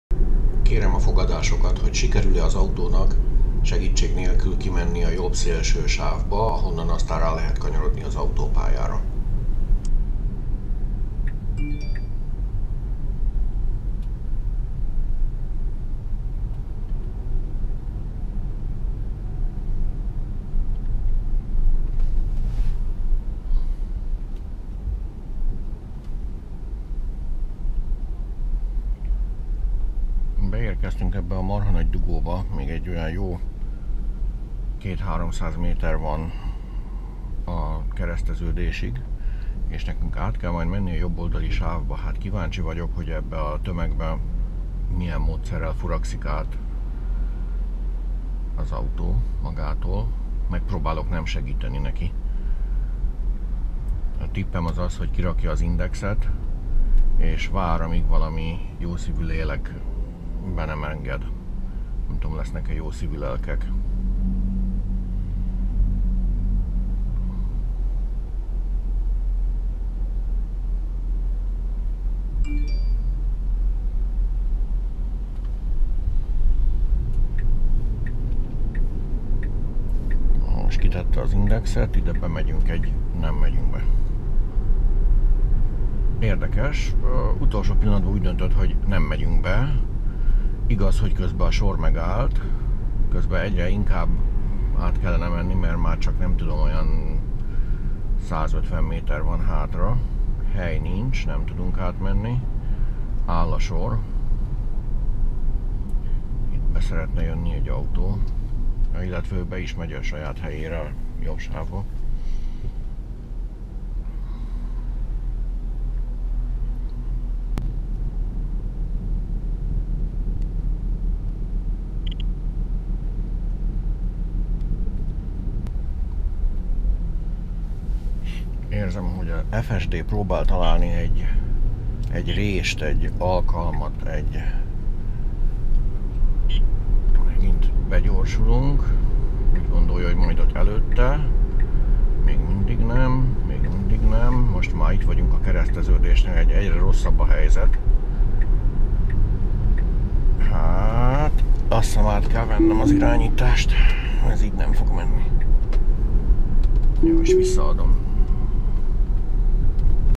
FSD 10.69.2.2 Can it change lane in high traffic?
Ezt a játékot már 10 éve űzöm: minden nap hazafelé ezen a helyen ki kell sorolni a külső sávba, hogy feljussunk az autópályára. A tökélyre fejleszett taktikám az hogy megyek a legbelső sávban aztán bunkó módjára az utolsó pillanatban átmegyek és bevágok valaki elé aki elbambult.

Na de mit csinál az FSD? Mennyire agressziv? A videóból kiderül.